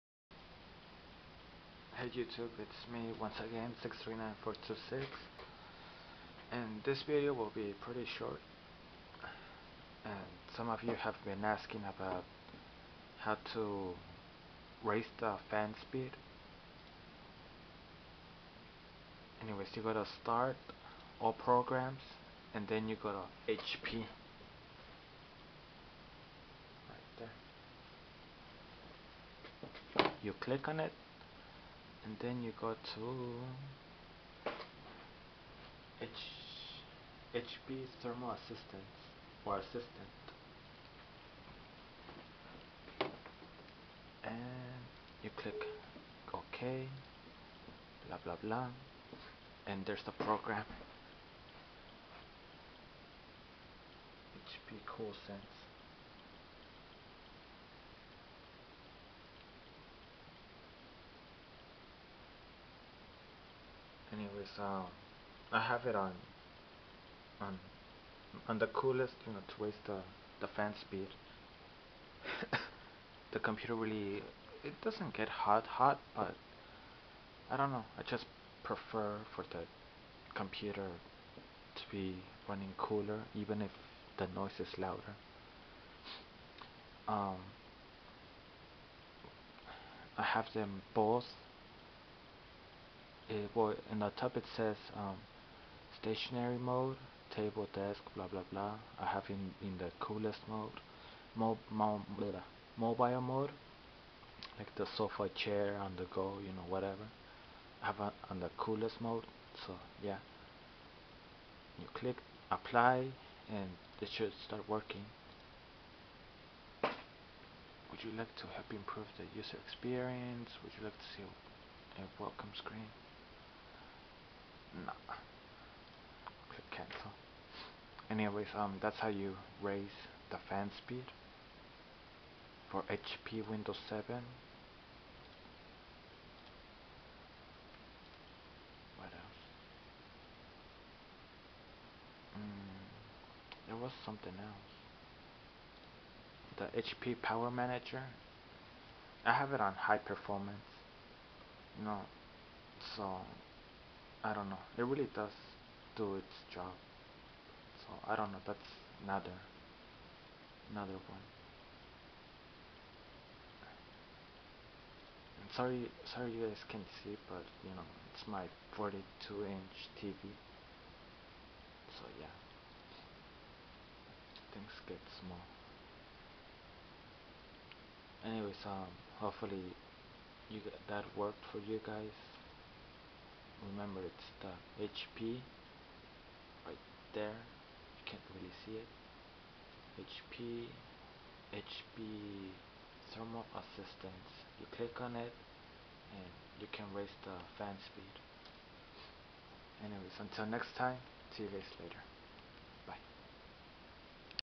How to raise fan speed, como subir velocidad de ventilador de PC
a new video just showing how to make the pc fan speed faster,